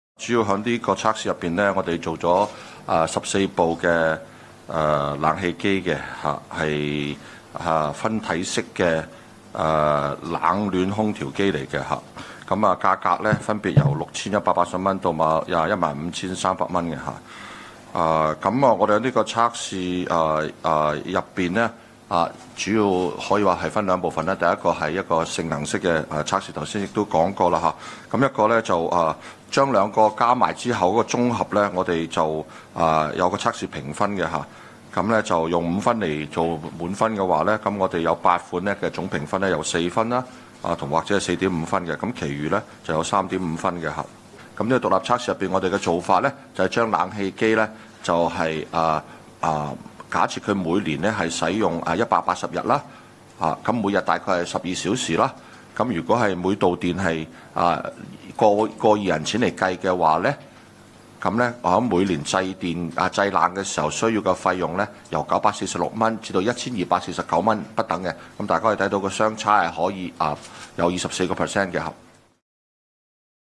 消委會：14款變頻冷氣耗電參差 電費差24%
炎炎夏日，不少市民都會開冷氣消暑，但又怕開太長時間電費會很貴。消委會抽查市面14款兩匹的變頻式分體冷暖空調機，發現耗電量相差高達20%，全年電費相差逾300港元，差距達至24%。

消委會抽查市面14款製冷量達兩匹，能源效益達一級的變頻式分體冷暖空調機，售價介乎6180至15,300元，聲稱製冷量由5至5.3千瓦，測試發現大部分樣本量得製冷量與聲稱大致脗合，當中「開利42QCEJ18VE/38QCEJ18VE」和「約克Y9ZH18(Y9EH18/Y9DH18)的量得製冷量比聲稱低2.8%及3.6%，但仍在強積性能源效益標籤計劃可容許的10%誤差範圍內。不過，消委會強調相信消費者普遍期望產品實際製冷量不會低於其聲稱低。

節能表現方面，製冷能源效率最高的「三菱電機MSZ-WG18VA-H1/MUZ-WG18VA-H1」比最低的「約克Y9ZH18(Y9EH18/Y9DH18)全年省電約20.1%，假設每部冷氣機每年開180日，每日開12小時，以每度電1.2港元計算，兩者電費分別為946港元和1249港元，相差達303港元，以較高電費者為基準計算，差距達到24%。

保養期方面，僅4款有2年保養，其餘只保養1年，而保養期內有10款會因為用戶位處偏遠地區而收取70至450港元上門檢查費。消委會促請代理商盡量延長保養期，提供較便宜的續保年費及維修費，鼓勵消費者繼續使用產品，減少浪費。

針對14款冷氣機同屬於一級能源效益標籤，但耗電量相差20%，消委會指機電署雖然近年收緊能源效益標籤要求，但一級標籤的電器之間的耗電差異範圍仍然很大，建議機電署考慮再細分能源效益等級，協助消費者選擇更節能的電器。